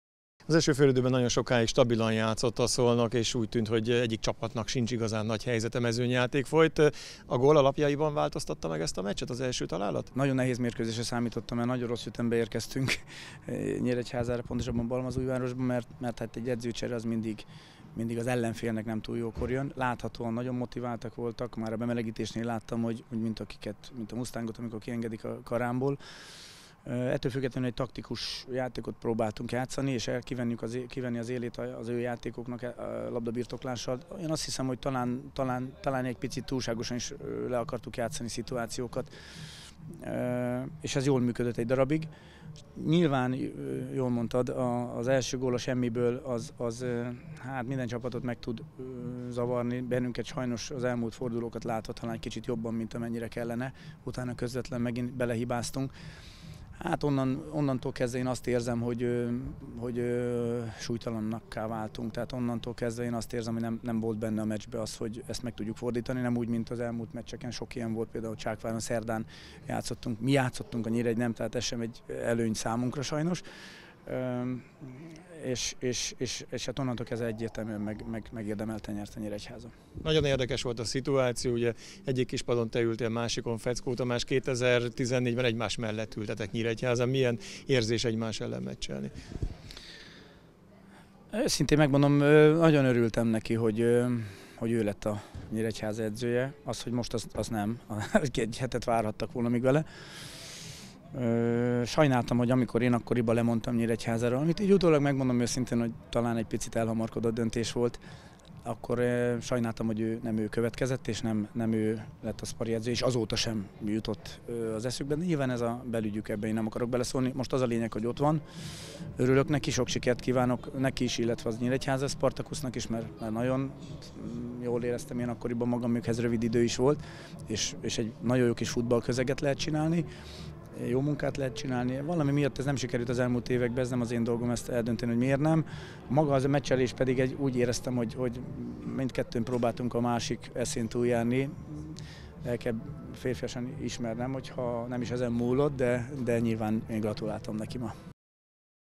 Szpari - Szolnok - Így látta a mérkőzést Csábi József
A 13. fordulót rendezték a Merkantil Bank Ligában.